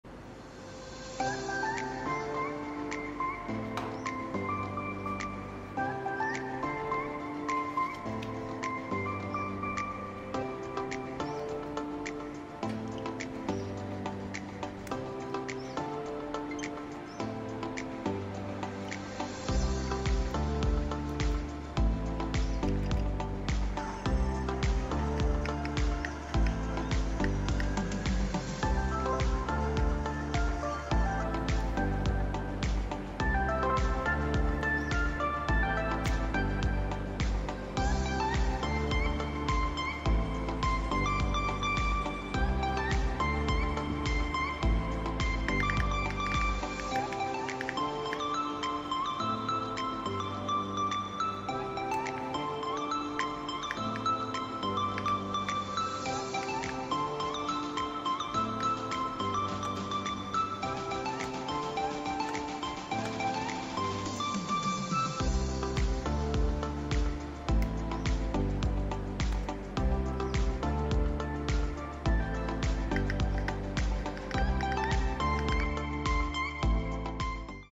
How to connect IINE Pocket Game Controller to different platforms?🧐
Gaming Fun in the Palm of Your Hand, Anytime, Anywhere!💙💗
Experience gaming with IINE Pocket Game Controller, no longer confined by platforms, immerse yourself in the charm of gaming and turn every control into a victory!😝